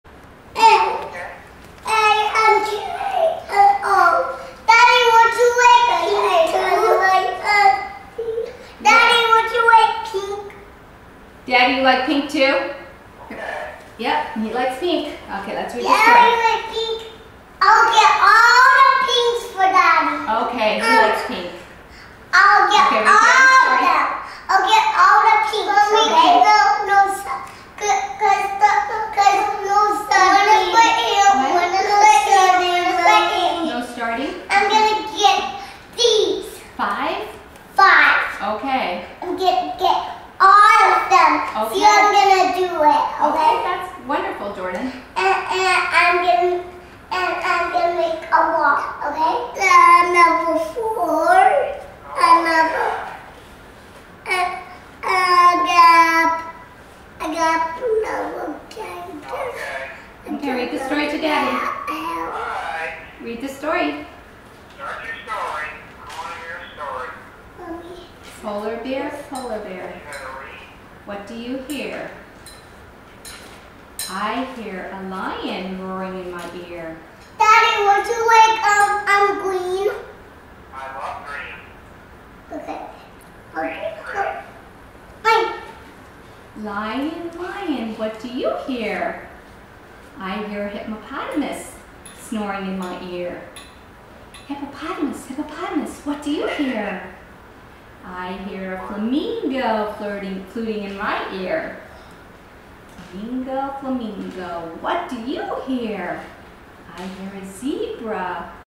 Justin Reading to Daddy on the phone
Justin likes talking to daddy on the phone and reading stories to him.